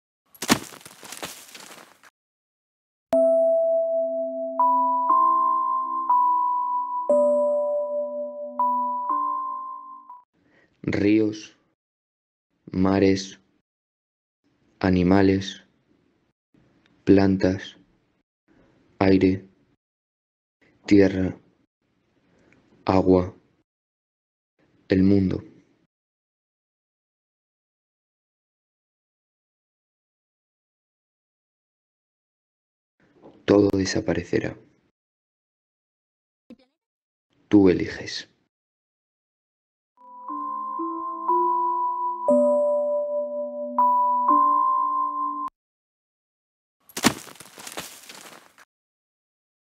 Tú eliges
Es poco lo que podemos hacer, pero si queremos que esto cambie, lo tenemos que hacer.

IES NAVARRO VILLOSLADA 4º ESO
Festival de Clipmetrajes de Manos Unidas